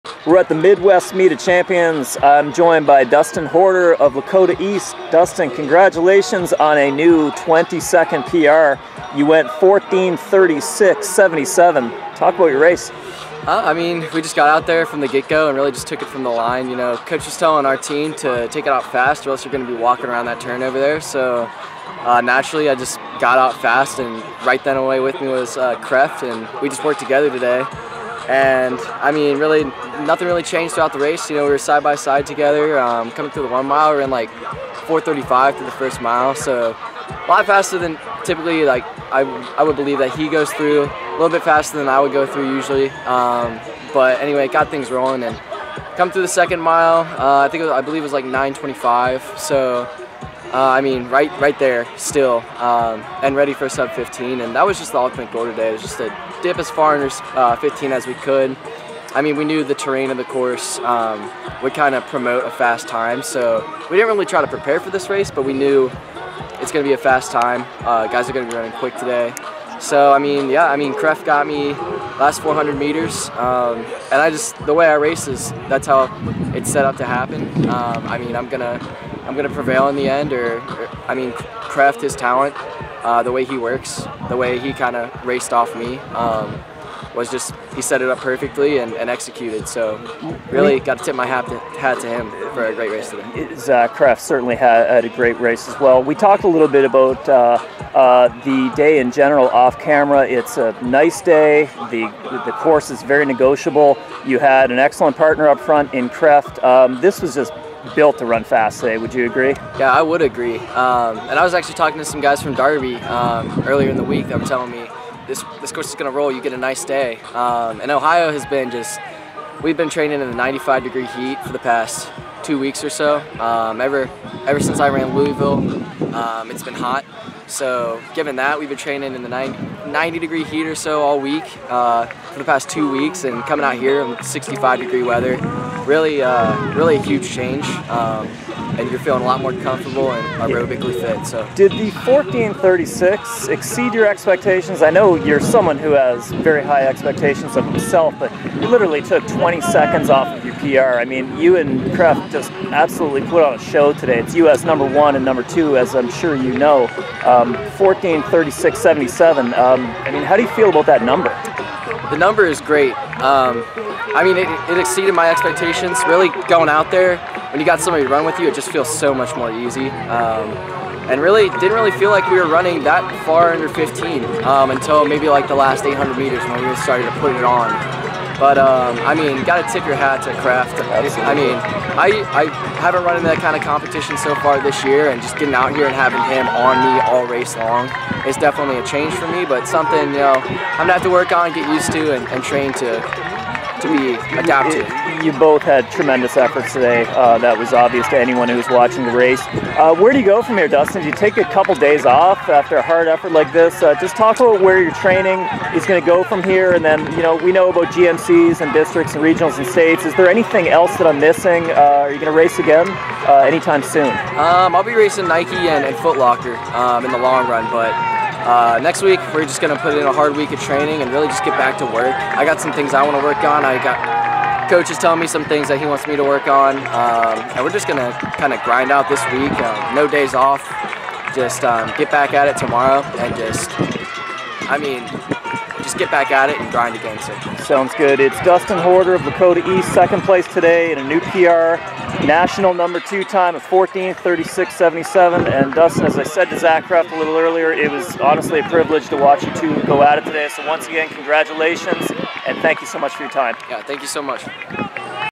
Dustin Horter After 14:36 5k
Dustin Horter ran a 14:36.77 for U.S. #2 in the same race as Zach Kreft at the Midwest Meet Of Champions!